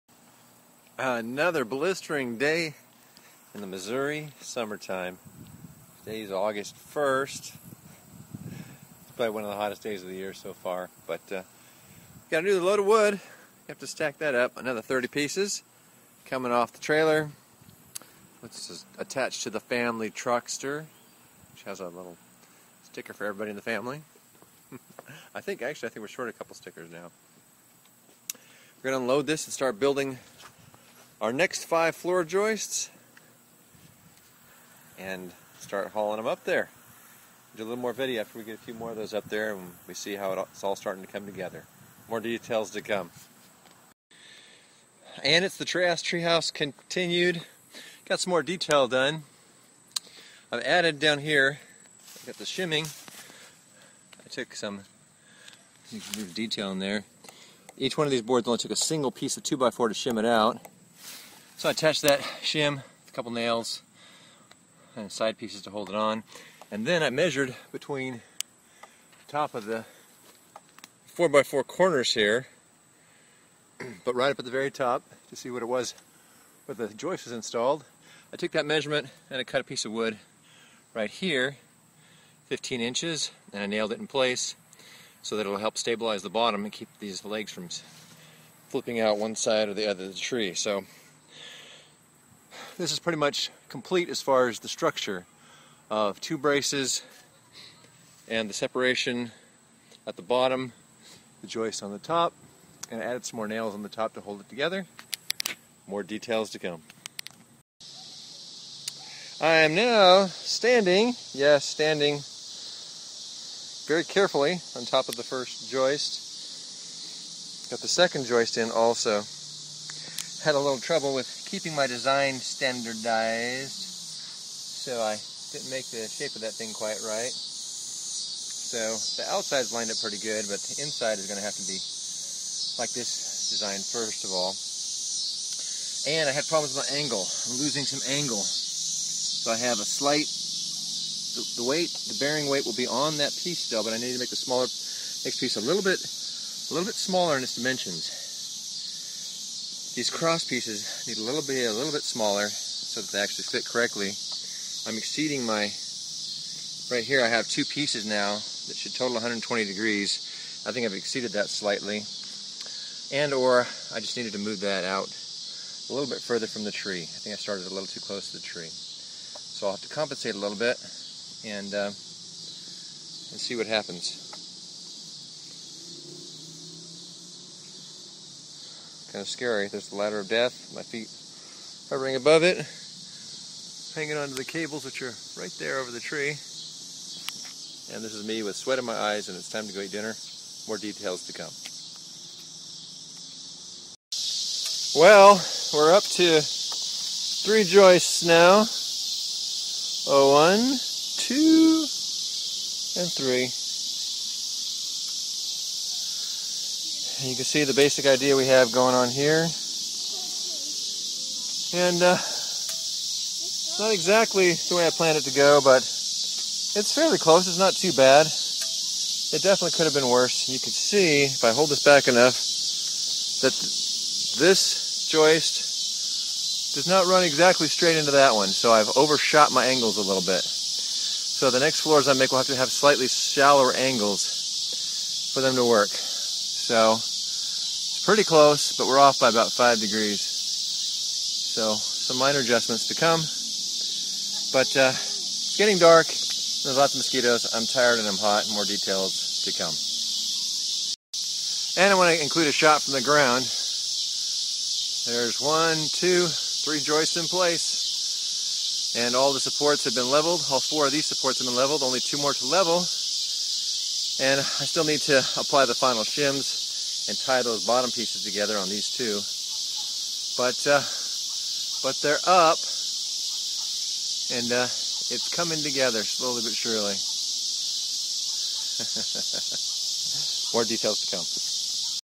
Trast Treehouse - Part 11
Floor joists 2 and 3 have been added. Discovered a problem with my math. The cross section of a 2x4 cut at a 30 degree angle is 1 and 3/4 inches, not 1 and 1/2, which caused my second joist to be misaligned near the tree by 1/2 inch and all 3 joists to slightly bigger than I originally planned. The adjoining joist for number 2 will be done last and I will do it by measuring the remaining space to make up for any of my math and/or cutting errors :) Not perfect but getting better! More details to come...